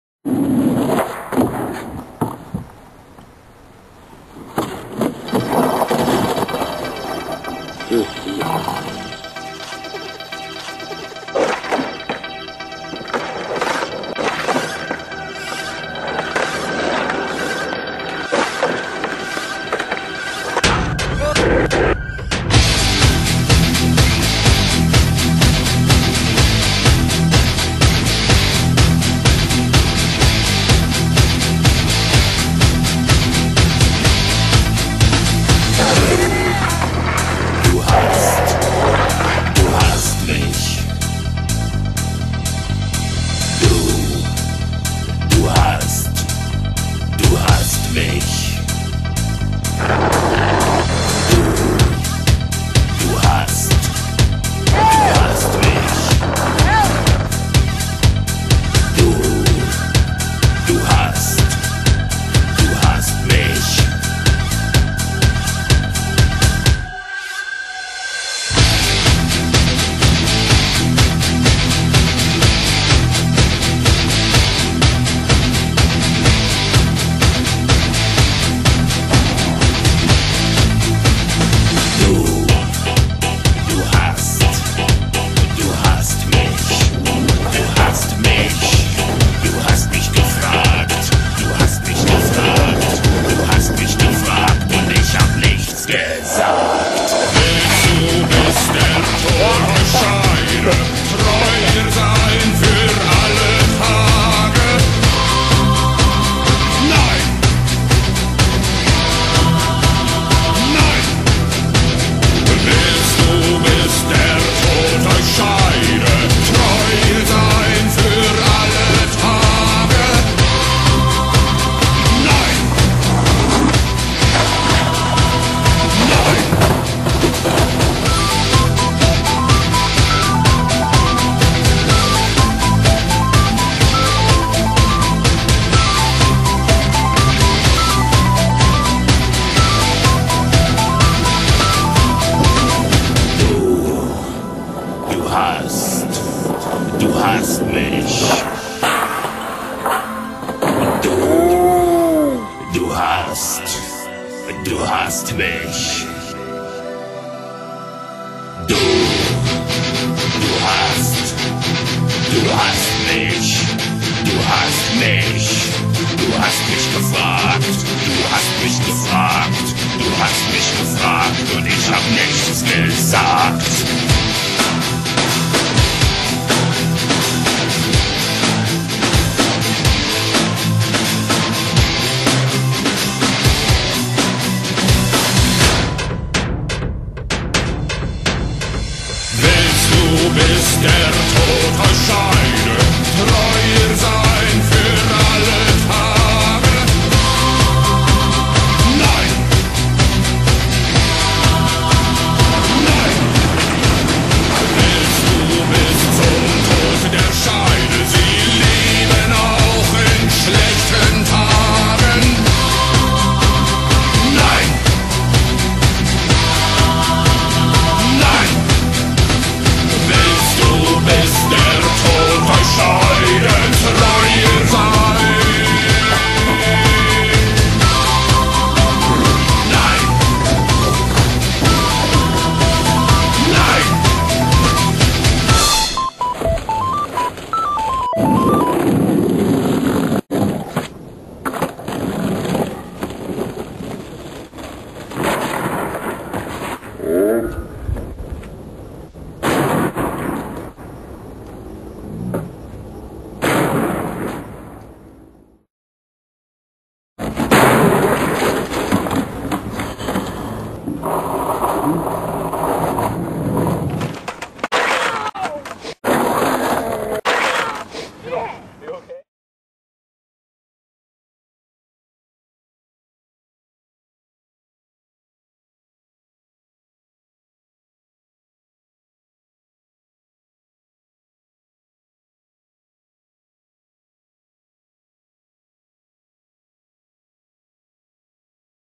Old Skateboarding with Justin. (followed by the most epic board break of all time)
Found a lost tape of random skating of my friend Justin from the early 2000's.

Thought it would be fun to do some old school skateboard vid editing techniques with splicing.

This video was never touched before now unfortunately.. enjoy! we had a lot of fun in those days.